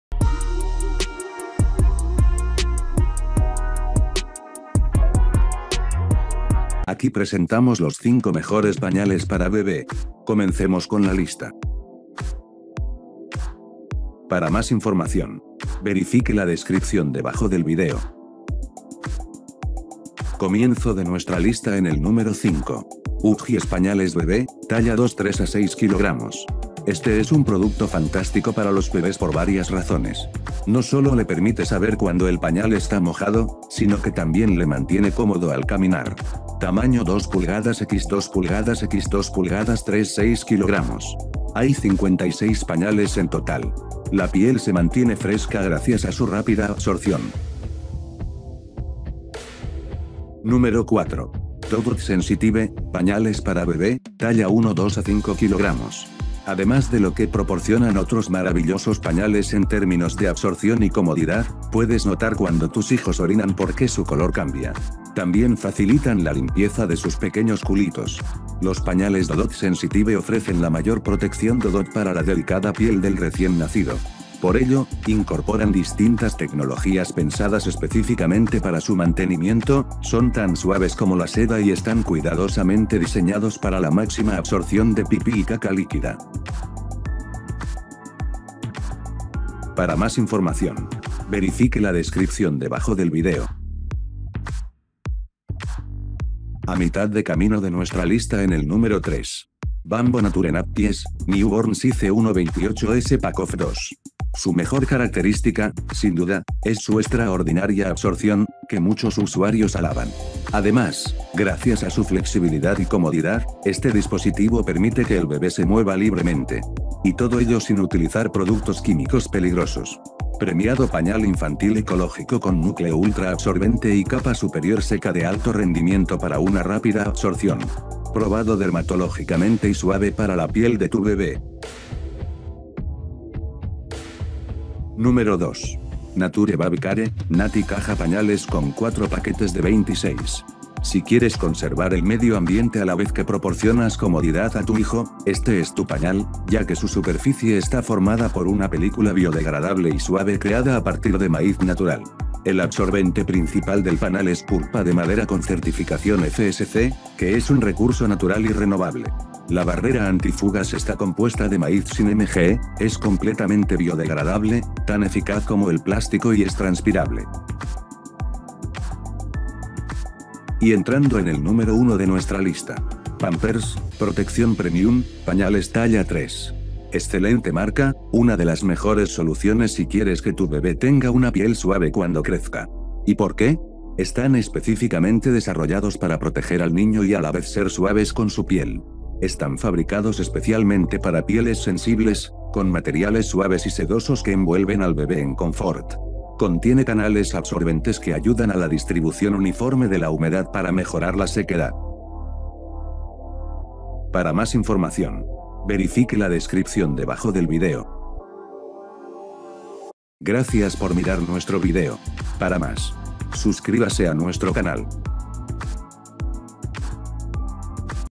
Los 5 Mejores Pañales para Bebé 2022
Aquí está la lista de Los 5 Mejores Pañales para Bebé.

01. Pampers - Protección Premium - Pañales Talla 3

02. Nature Babycare - Naty Caja Pañales con 4 paquetes de 26

03. Bambo Nature Nappies - New Born Size 1 28s (Pack of 2)

04. Dodot Sensitive - Pañales para bebé, talla 1 (2-5 kg)

05. Huggies Pañales Bebè, Talla 2 (3-6 kg)